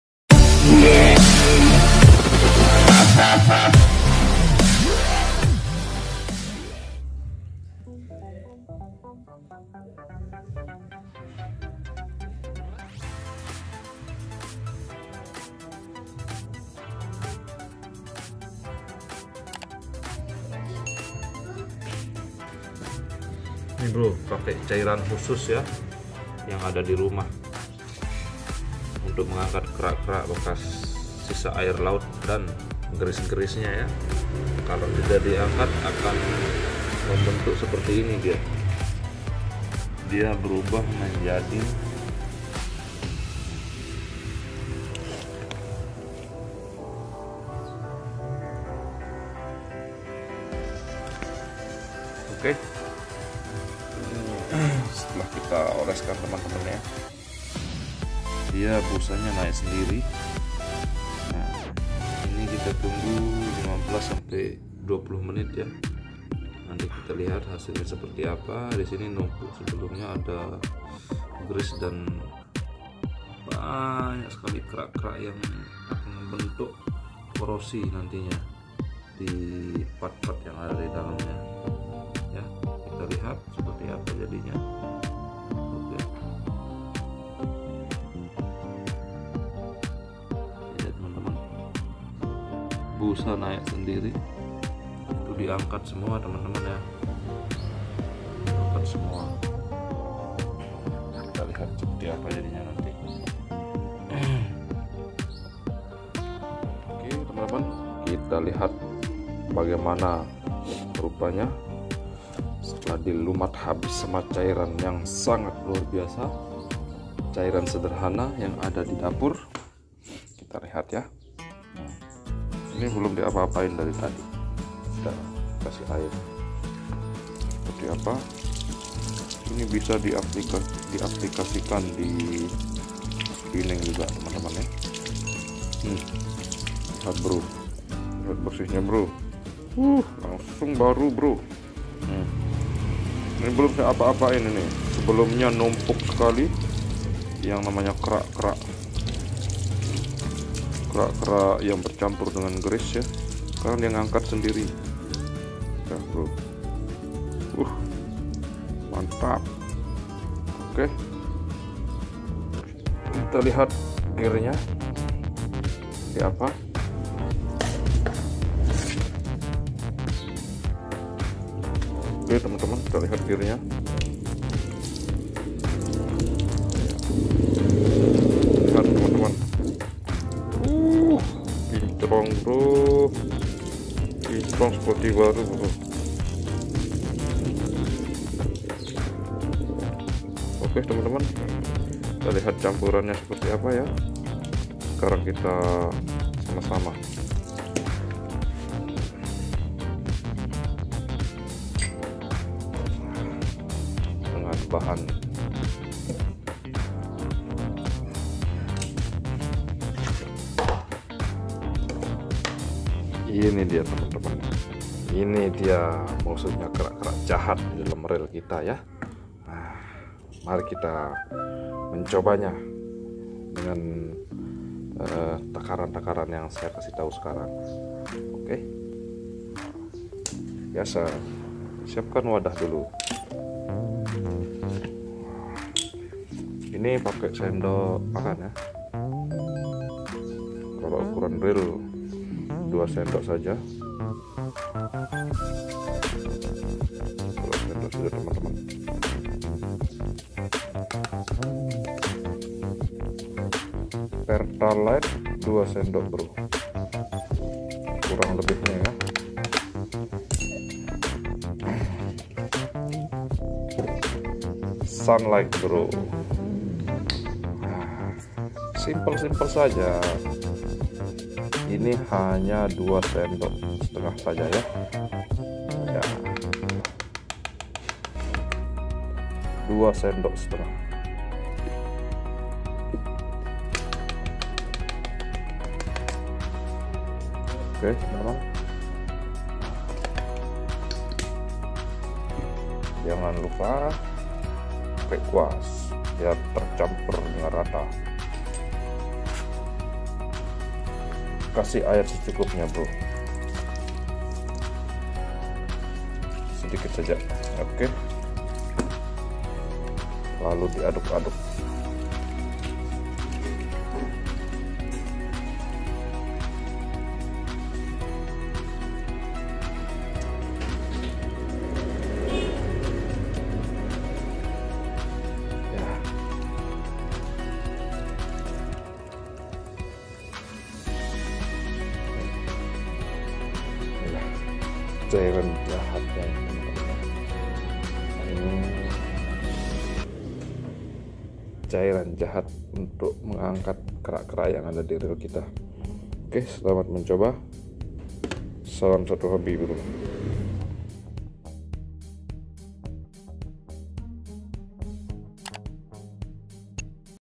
Cara membersihkan rell overhead rell pancing dan kerak membandel_cairan_khusus#di_dapur
Oke teman hari ini libur mancing dulu
Kali ini saya berbagi pengalaman cara membersihkan rell overhead dan rell pancing maupun spinning karna kerak membandel pada overhead kalau di biarkan itu yang menyebabkan rell overhead cepat korosi dan rusak
Karena peralatan memancing perlu perawatan juga..

Mancing#dan_tutorial_sederhana